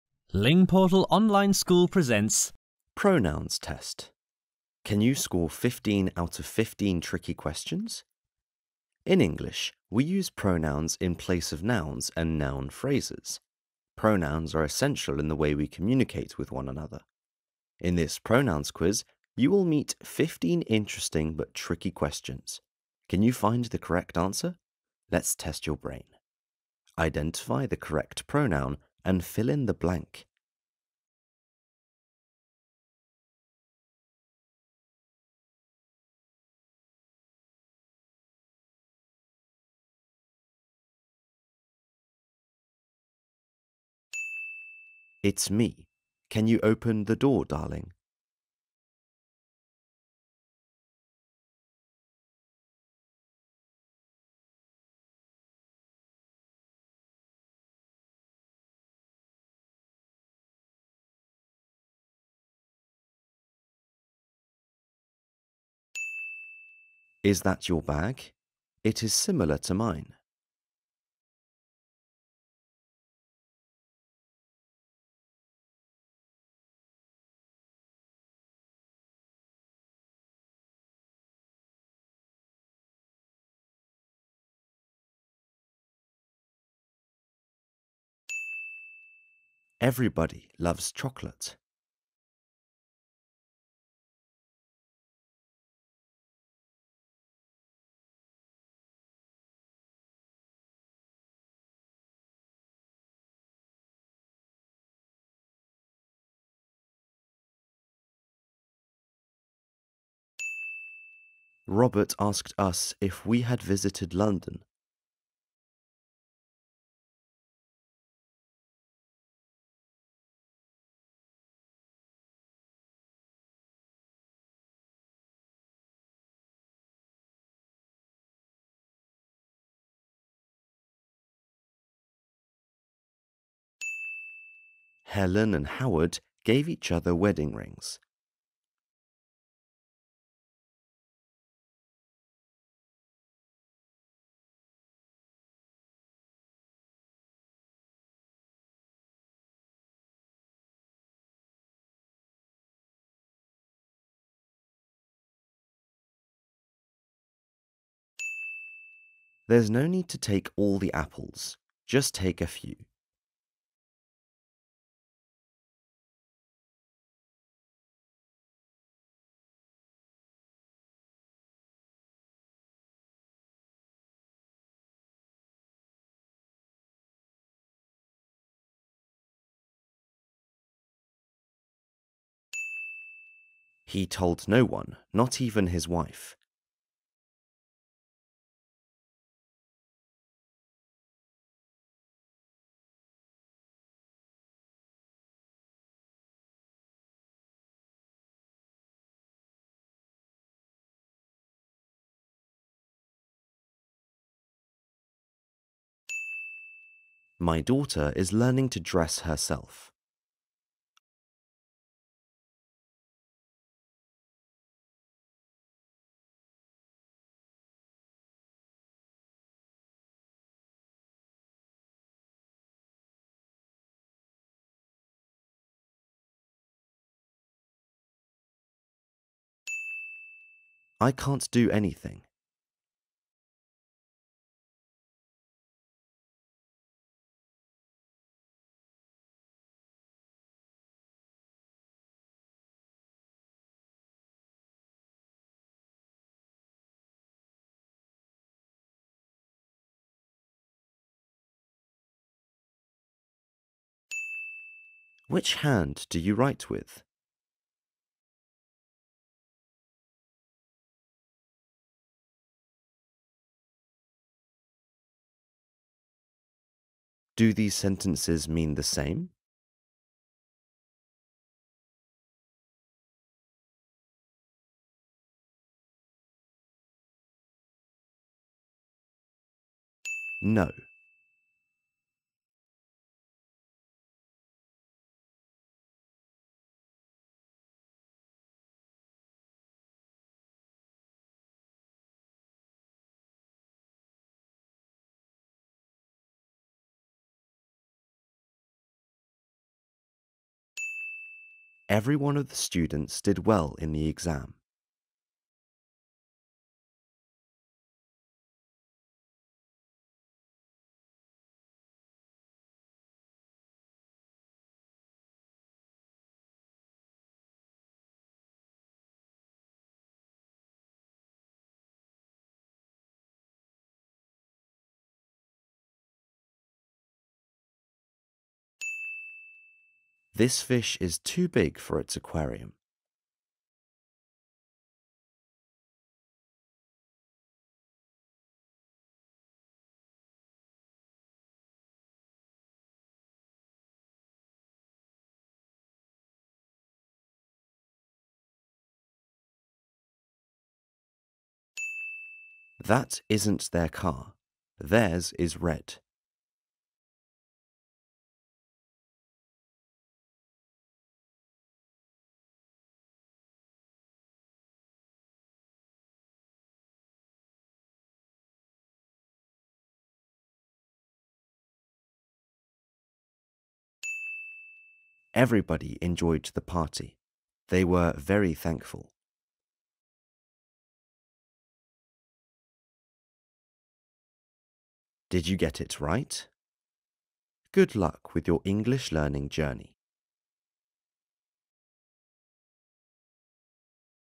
Pronouns Test - How many of these 15 tricky questions do you know? – English Grammar Test
In English, we use pronouns in place of nouns and noun phrases.  Pronouns are essential in the way we communicate with one another.
In this Pronouns Quiz you will meet 15 interesting but tricky questions – can you find the correct answer?
let’s test your brain!
Identify the correct pronoun and fill in the blank.
Good luck!

Stay with us and enjoy your English learning.

Practice makes perfect!  Stay with us and enjoy your English learning.